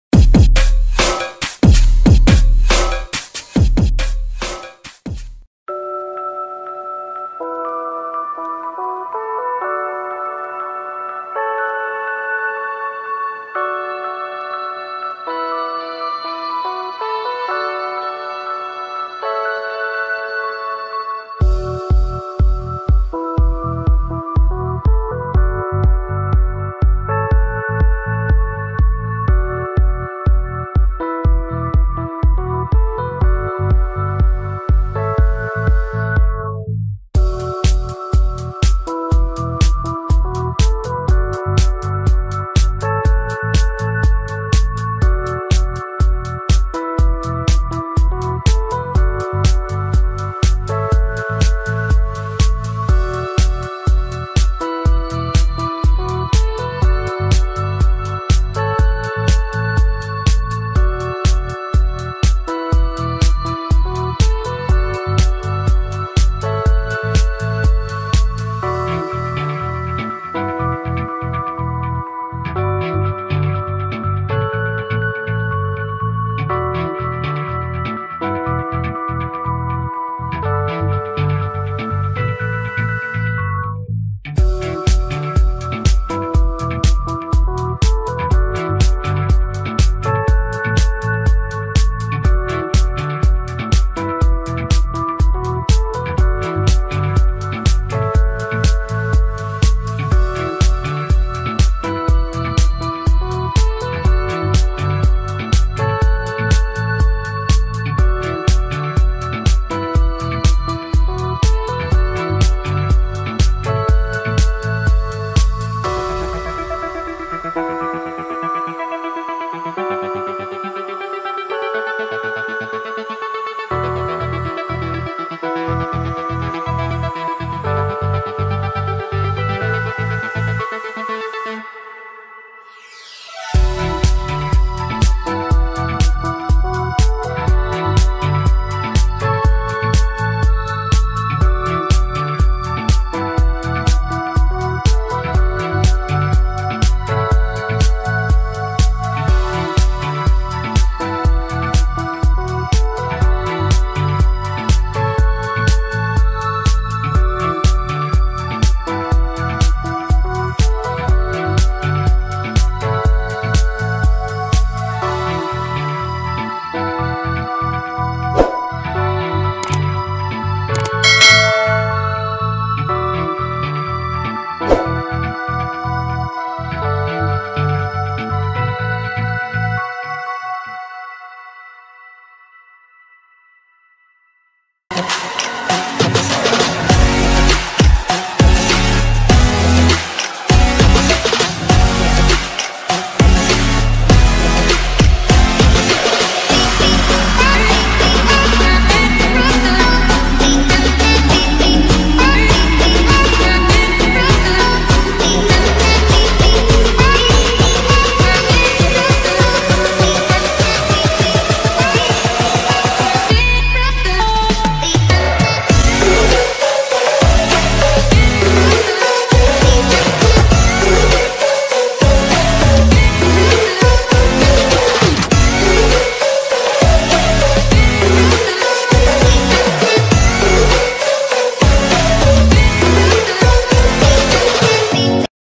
DIBUJANDO A IRON MAN!! | Jonathan Art
Bienvenidos a un nuevo video, hoy vamos a dibujar a Iron Man espero que les guste.